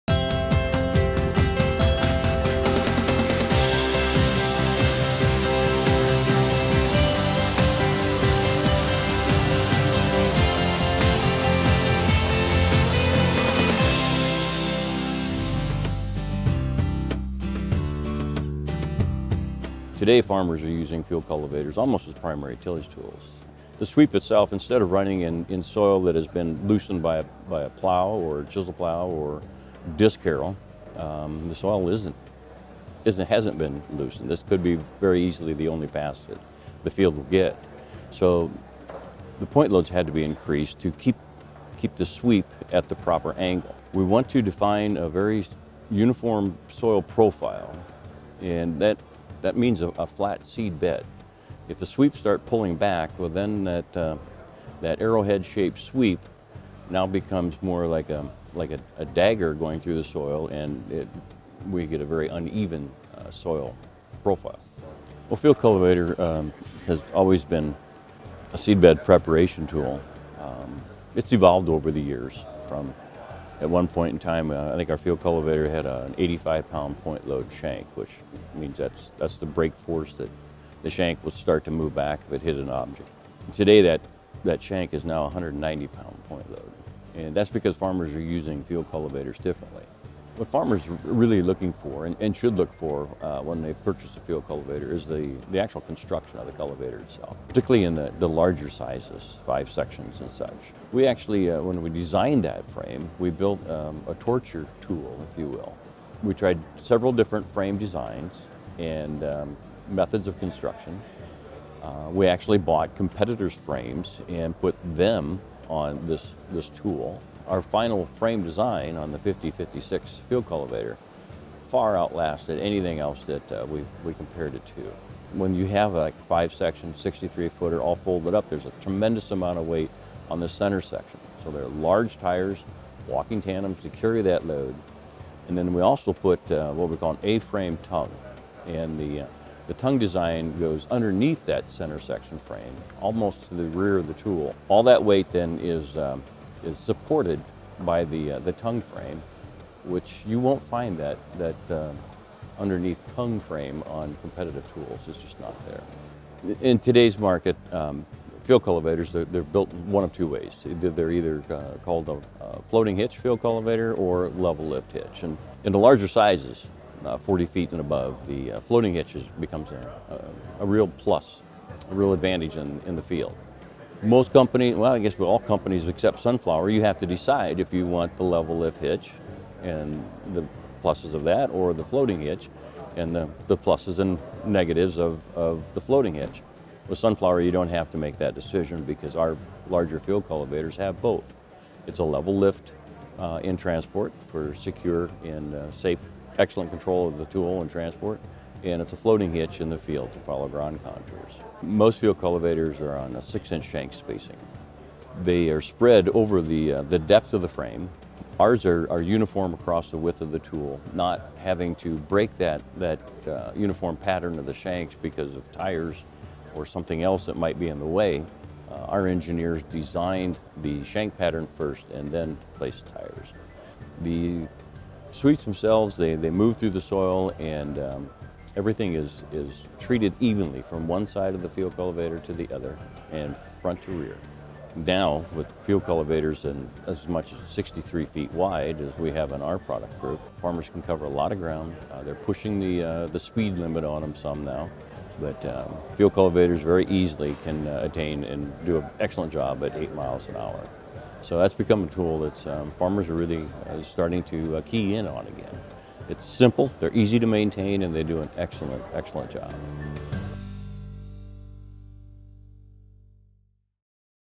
Sunflower® Field Cultivators - All-Purpose Productivity
When it comes to field cultivators, today’s farming operations demand both brawn and precision. Sunflower’s industry-leading engineering and manufacturing capabilities deliver on both counts, maintaining sweeps at the proper angle, undercutting completely across the width of the tool, and helping producers create a uniform seedbed.